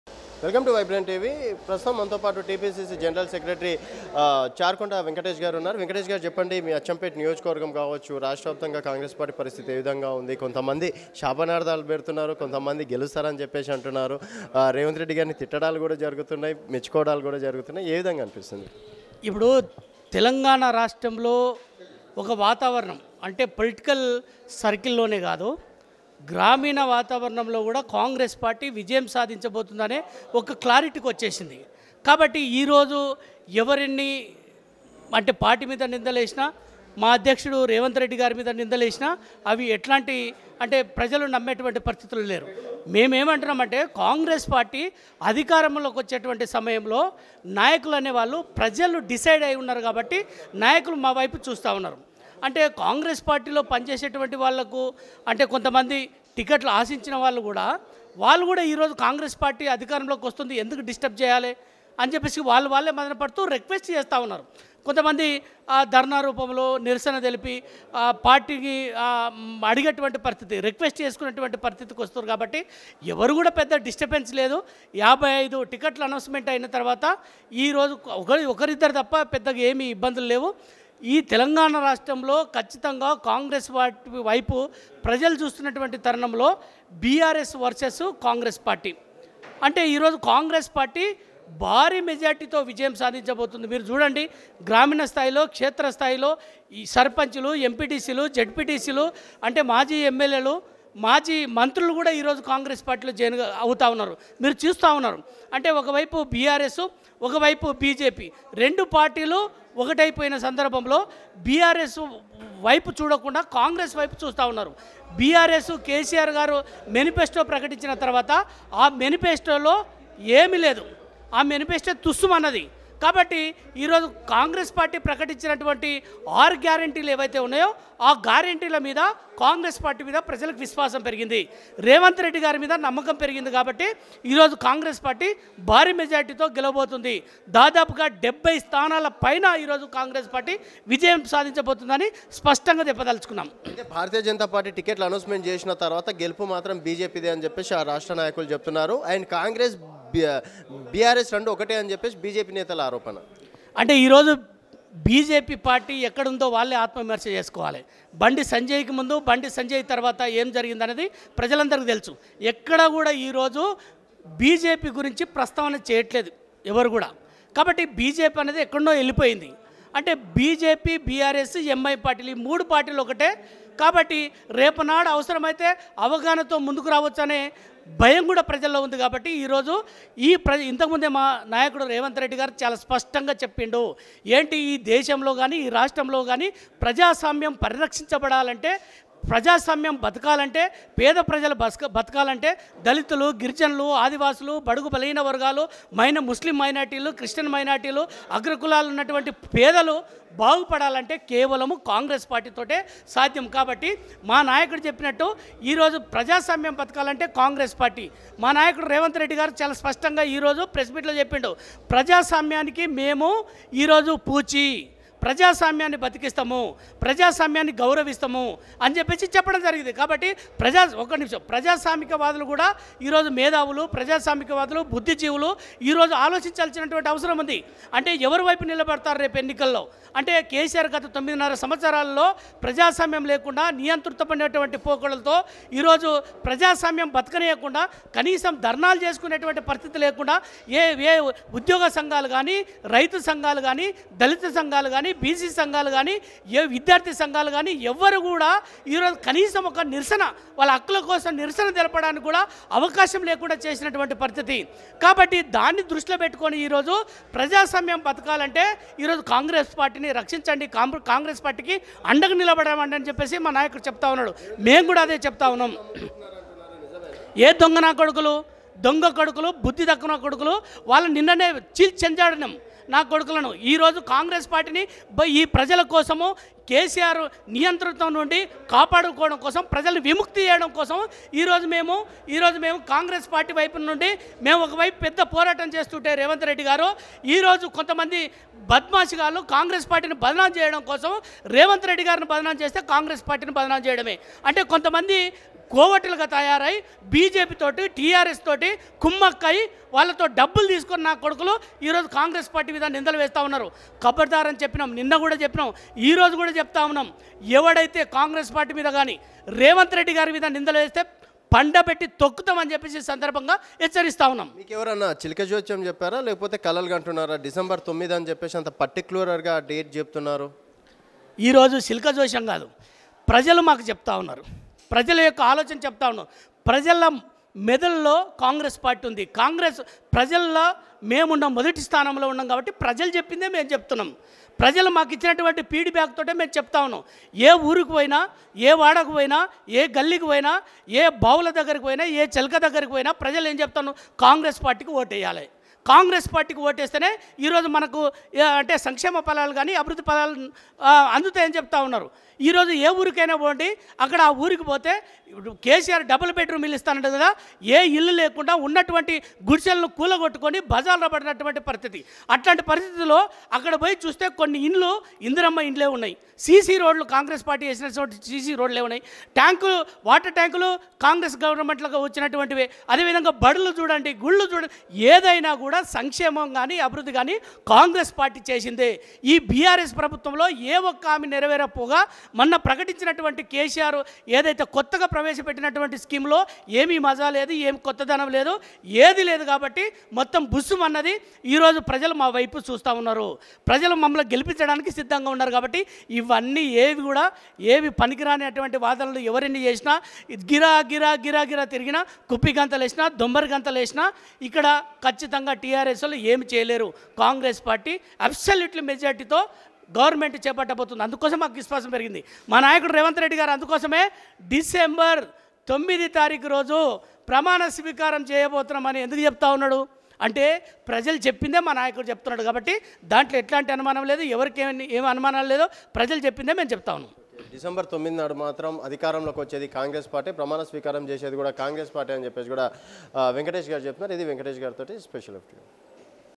రేవంత్ ను ఎమన్నా అంటే నాలుక చీరేస్తాం | Charukonda venkateshReddy | Telangana Election 2023 | CM KCR
Watch  Here :- రేవంత్ ను ఎమన్నా అంటే నాలుక చీరేస్తాం | Charukonda venkateshReddy | Telangana Election 2023 | CM KCR